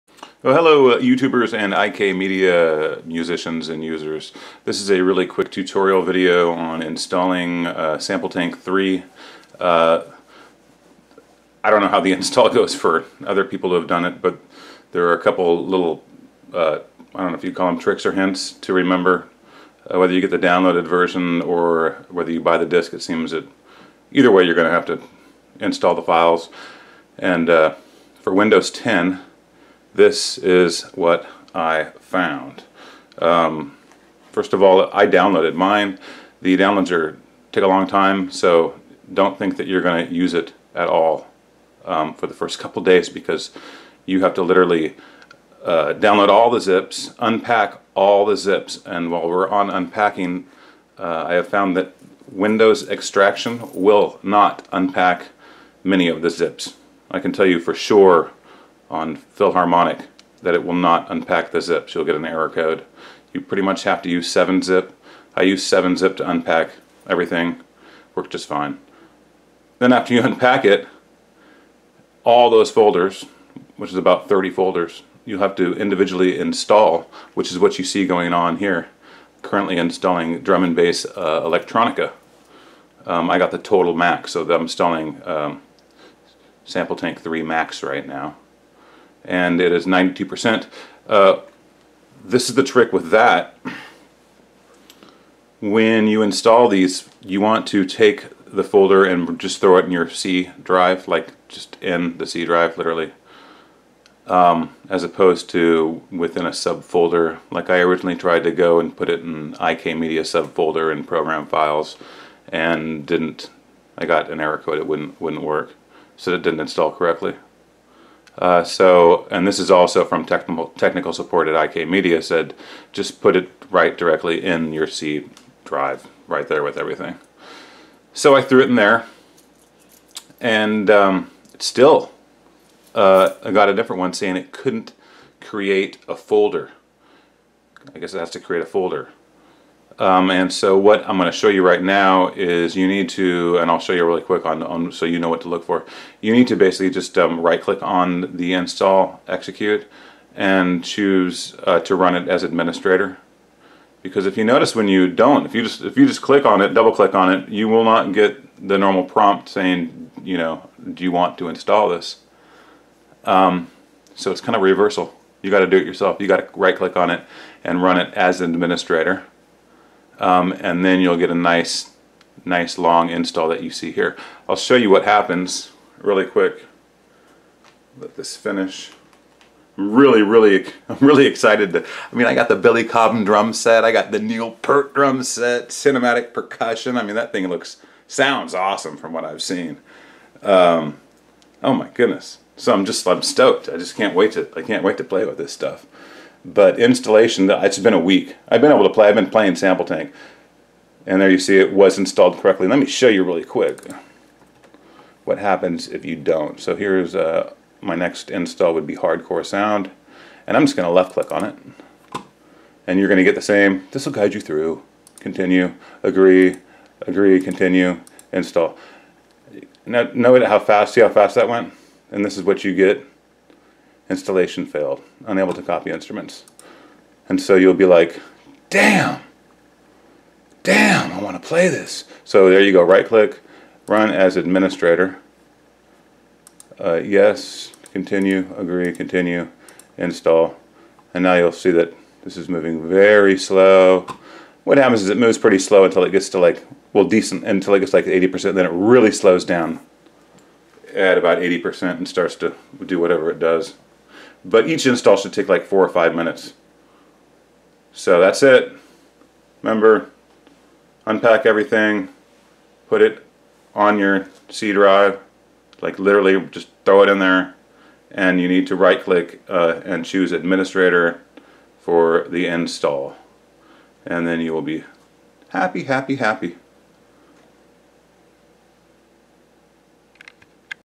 Installing Sample Tank 3
What to expect when installing Sample Tank 3